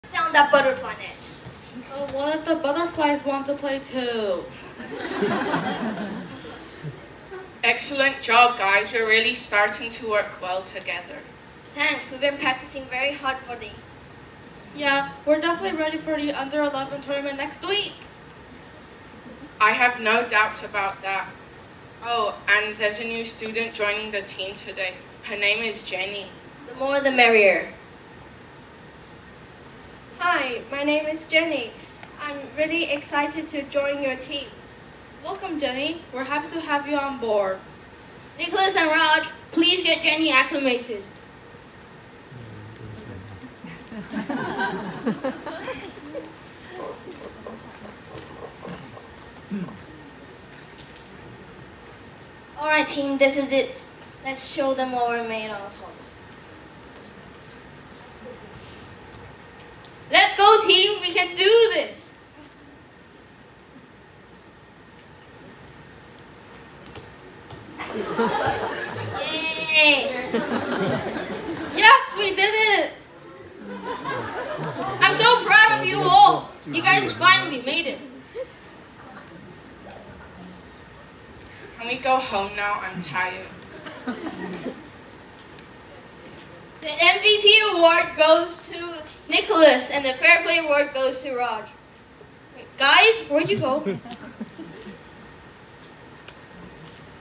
Soccer Drama
The kids performed a short drama showing the team spirit and welcome style for a new student